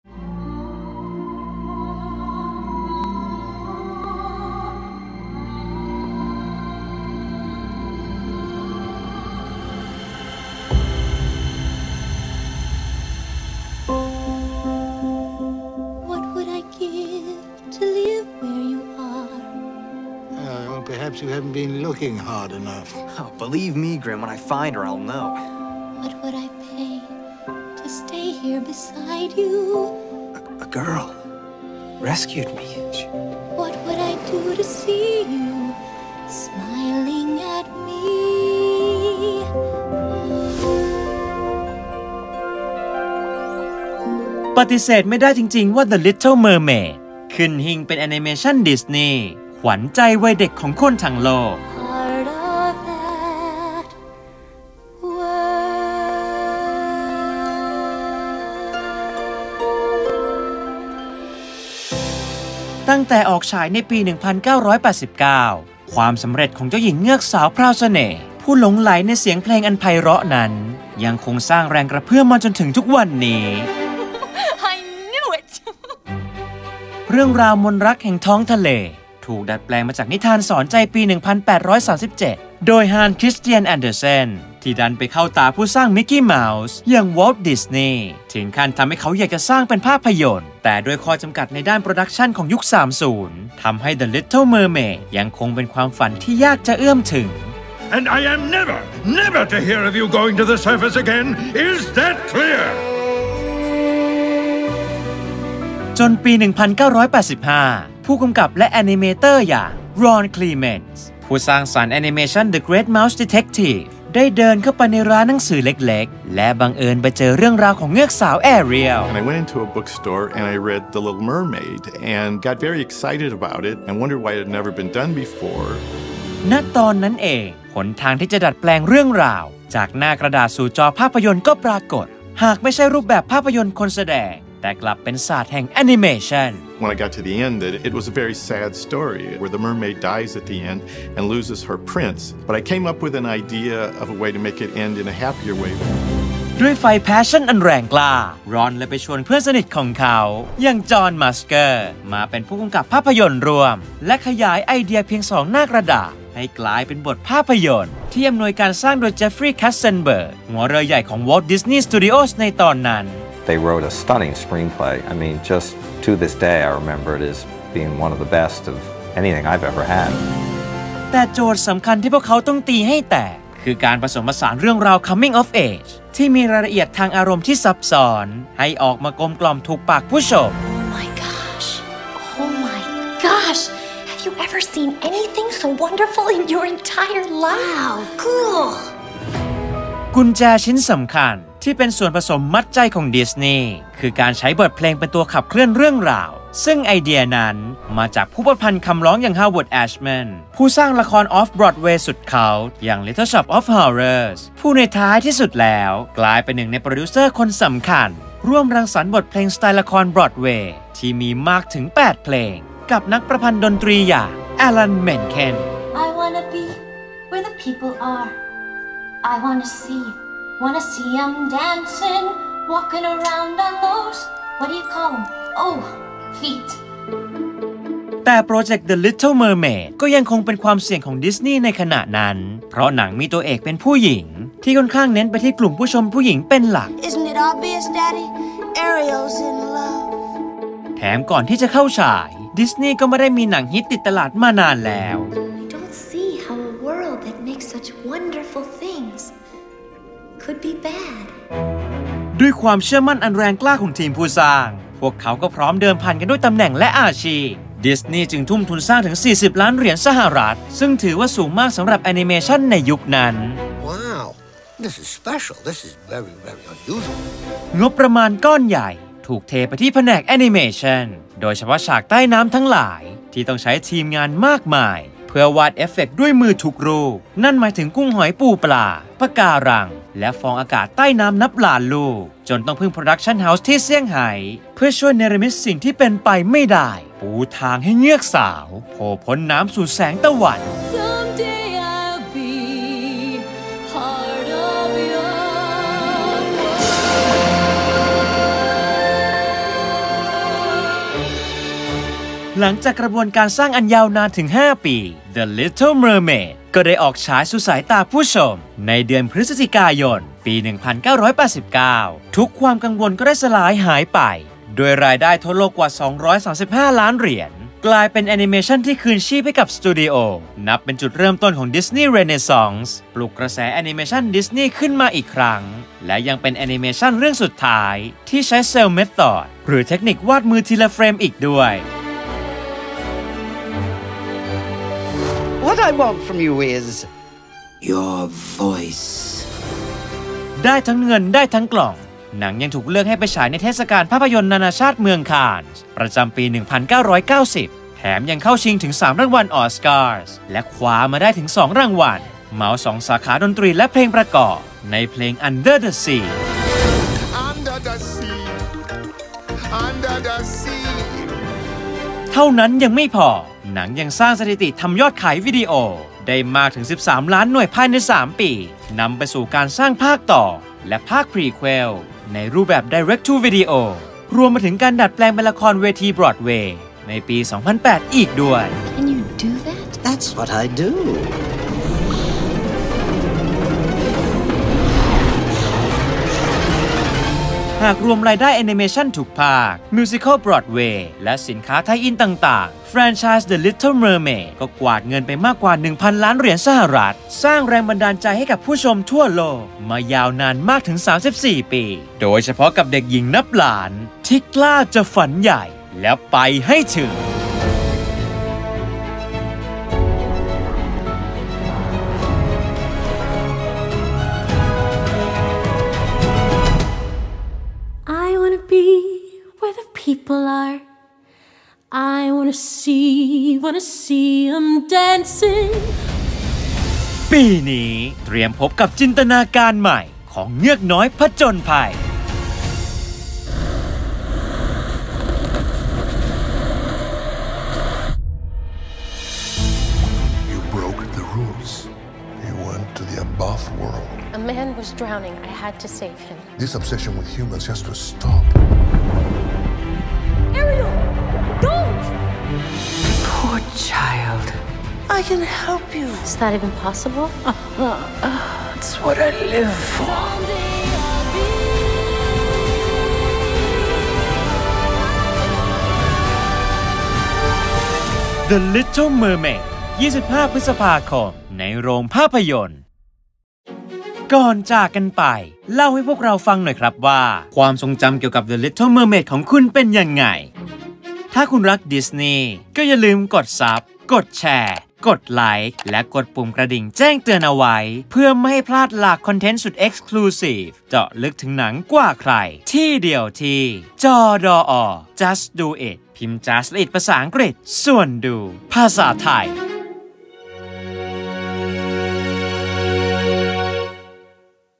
The Little Mermaid (1989) สุดยอดแอนิเมชั่นที่ทุกหัวใจหลงรัก | JUSTดูIT.
ย้อนกลับ สู่ วันวานอันหอมหวาน
ตำนานแอนิเมชั่นที่ทุกหัวใจหลังรัก
จุดเปลี่ยนครั้งสำคัญของ Walt Disney

เตรียมพร้อมก่อนพบกับการ "ตีความใหม่"
สู่ ภาพยนตร์ Live Action ฟอร์มยักษ์แห่งปี

The Little Mermaid เงือกน้อยผจญภัย
25 พฤษภาคม ในโรงภาพยนตร์